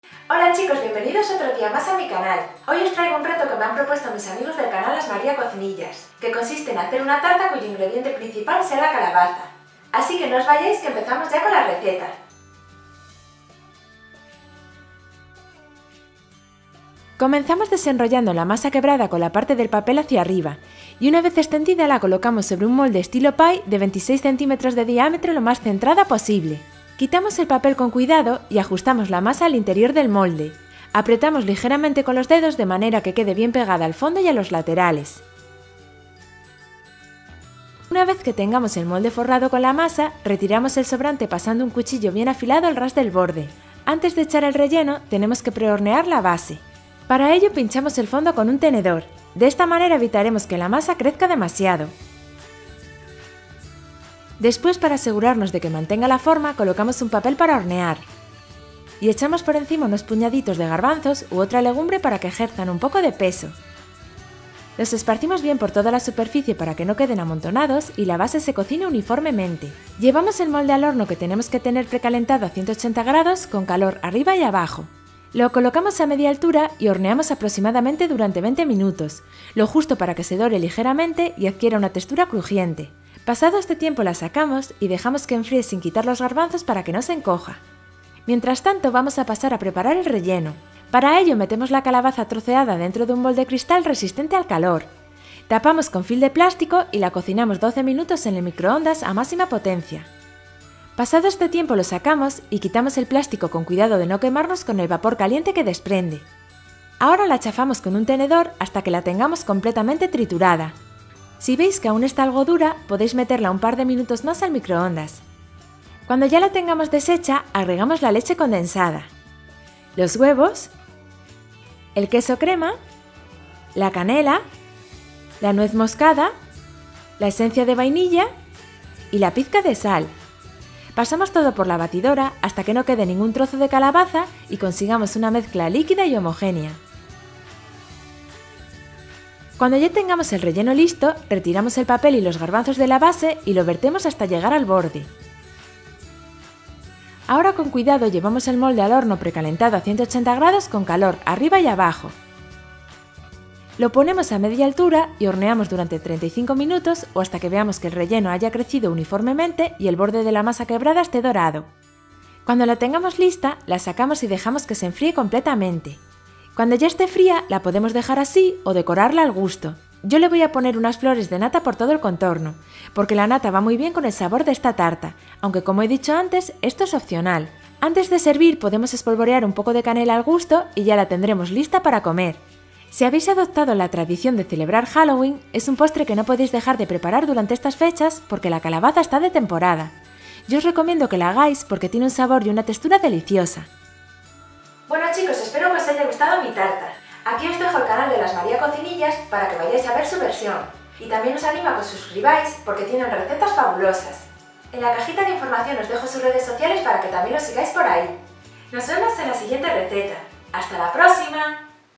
Tarta de Calabaza y Leche Condensada (Pumpkin Pie) | Reto con Las María Cocinillas
En esta videoreceta os enseño cómo hacer Tarta de Calabaza y Leche Condensada, una receta súper fácil para este halloween. Es un reto que nos hemos propuesto con el Canal Las María Cocinillas. Espero que os gusten.

Ingredientes:
1 masa quebrada refrigerada
600 gr de calabaza
400 gr de leche condensada
50 gr de queso crema
3 huevos talla M
1 cucharadita de canela molida
1 cucharadita de nuez moscada molida
1 cucharadita de esencia de vainilla
¼ de cucharadita de sal
Nata montada para decorar

Batidora de vaso ➜
Batidora de mano ➜
Molde de silicona (23 cm) ➜
Molde de aluminio (18 cm) ➜
Sartén antiadherente (26 cm) ➜